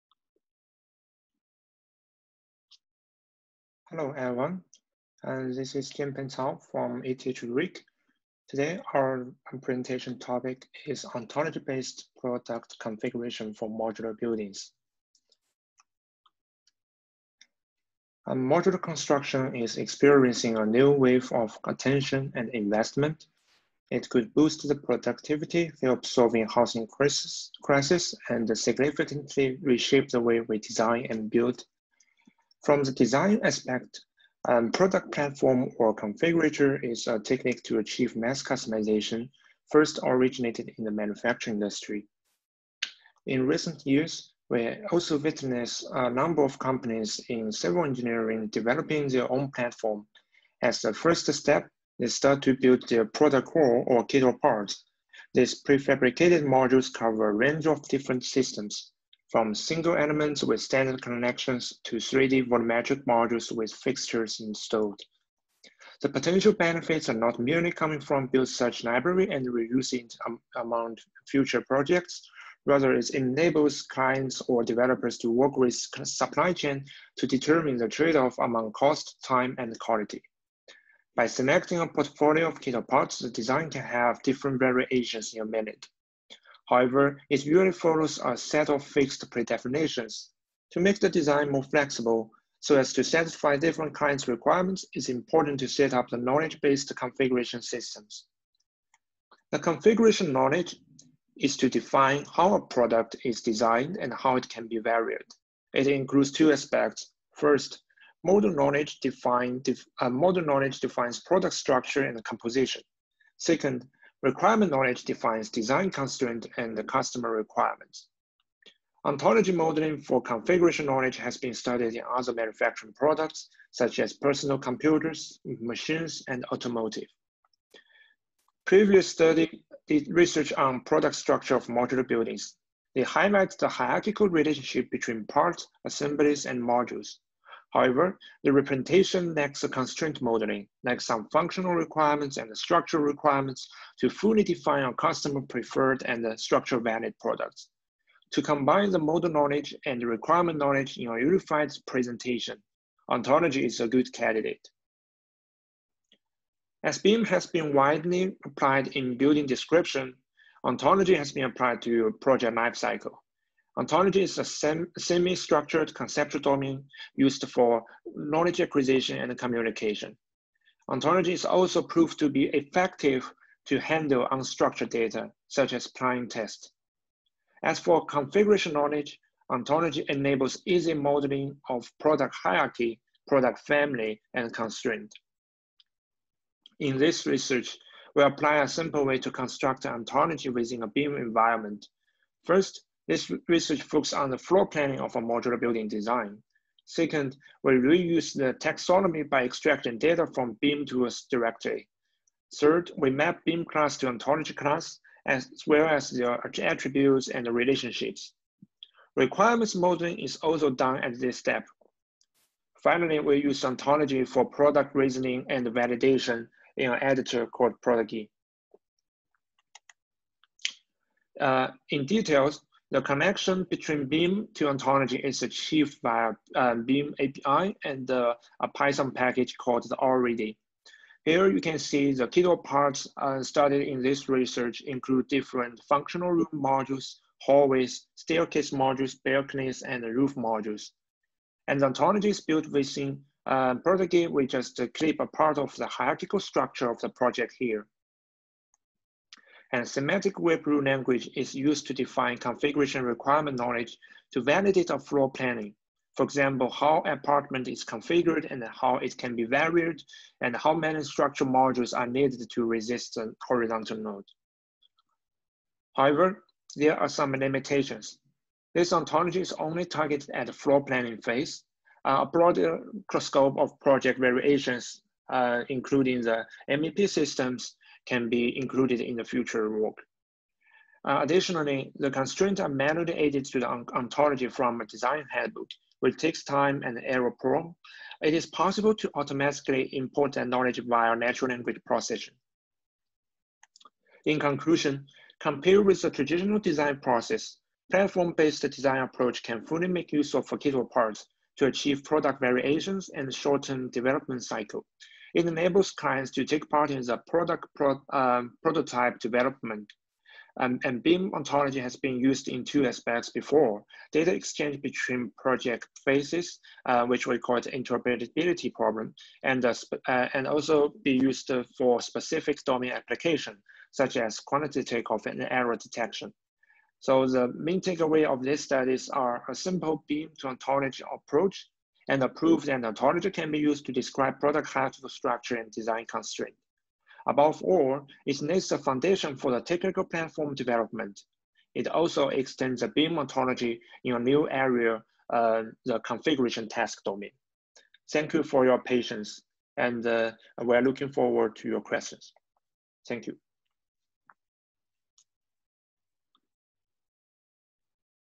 2020 ISARC | Jianpeng Cao | Ontology-based Product Configuration for Modular Buildings
This presentation was originally given at the 37th International Symposium on Automation and Robotics in Construction (ISARC 2020).

Ontology-based Product Configuration for Modular Buildings
Jianpeng Cao, Daniel M. Hall

Abstract: To support mass customization for industrialized construction, researchers and industry have adopted product configuration strategies. Based on the theory of modularization, pre-defined module libraries can be configured into feasible construction projects. However, the digital representation of configuration knowledge is understudied. Current Building Information Modeling (BIM) product modeling does not enable automatic generation and reasoning of configuration solutions for new products. This study proposes an ontology-based strategy to configure modular buildings. The proposed BIM-to-ontology workflow collects and formalizes configuration constraints. In particular, we elaborate on how those constraints can be modeled within the ontology in a modular building case. The ontology maintains the product hierarchy needed for product assembly. Such ontology development can support the rapid development of new products with customized variations. The paper identifies the limitations of the current product modeling approach. The proposed ontology can act as a foundation for BIM-based product platform development and increase the use of mass customization for industrialized construction.